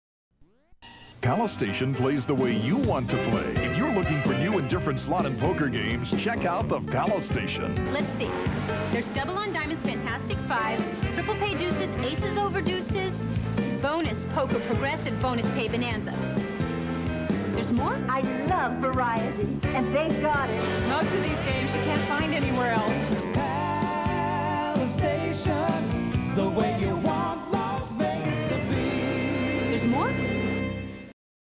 Palace Station Las Vegas 1994
1994 Commercial for the Palace Station located on US-15 and Sahara, Las Vegas, NV. This spot aired in march of 1994, when the only other station was the newly built "Boulder Station."
Many more would follow in the near future, including "Sunset, Texas, Red Rock, and Aliante."